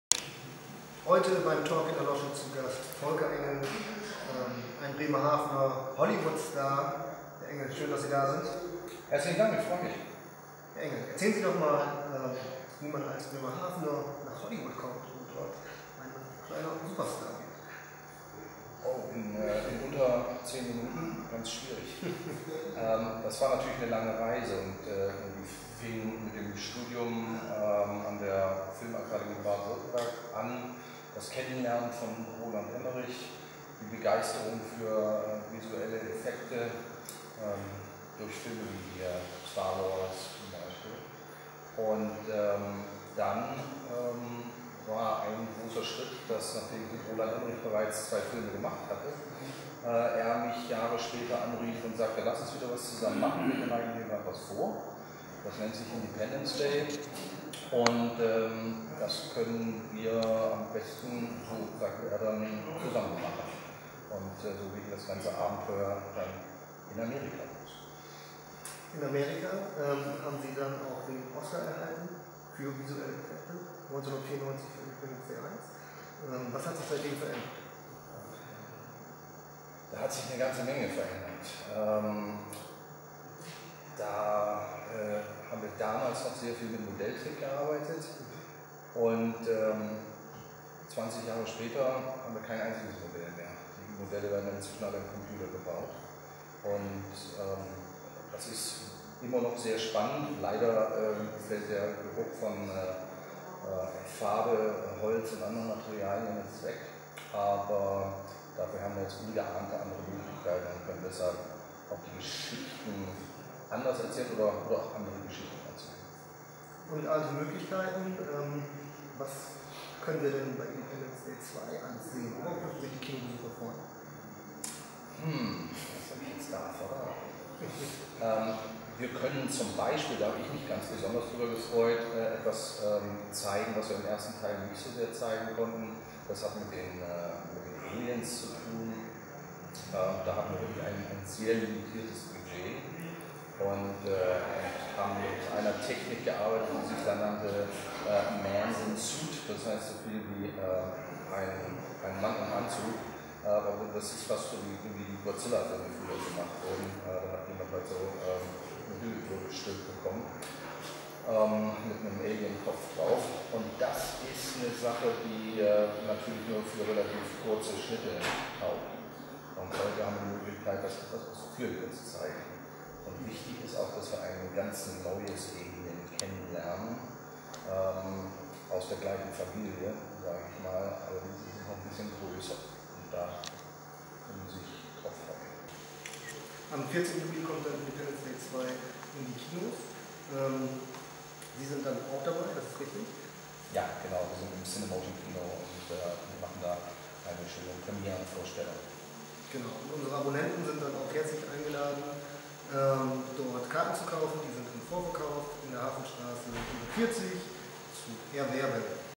Volker Engel über seinen Weg von Bremerhaven nach Hollywood
Einer der berühmtesten Bremerhavener ist zu Gast beim Talk in der Losche: Volker Engel. Er ist Co-Produzent von Independence Day 2 und hat für Independence Day 1 den Oscar in der Kategorie Visual Effects bekommen.